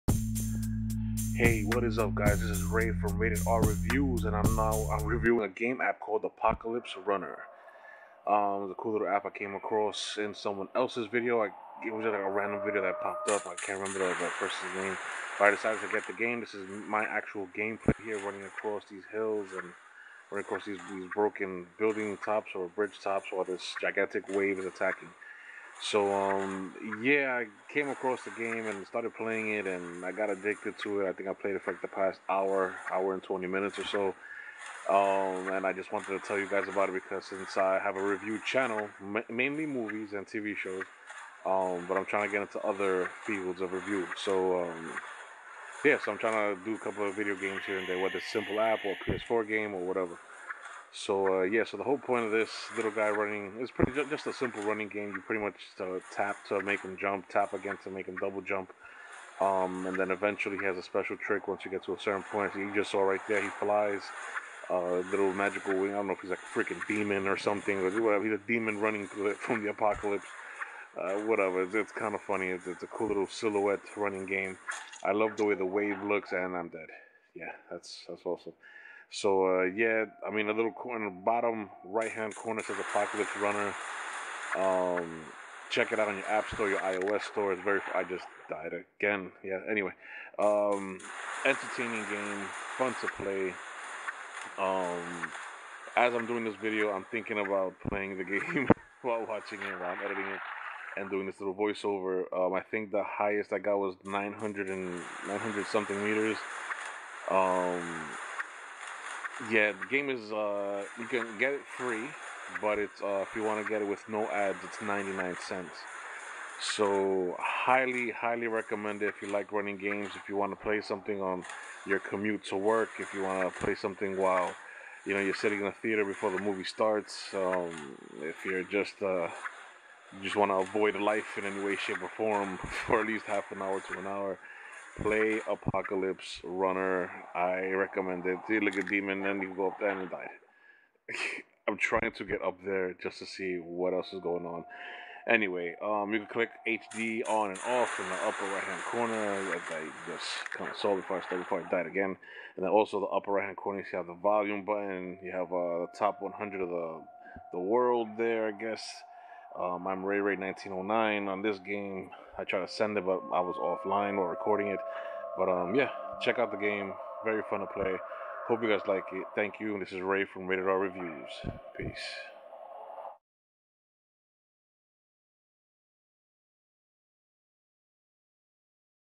Apocalypse Runner Review
Android apps are definitely abundant within the digital world. Copying eachother here and there where ever they can to make a buck... So why not review those that don't suck... I literally played this game for an hour straight... Dam near cracked my phone...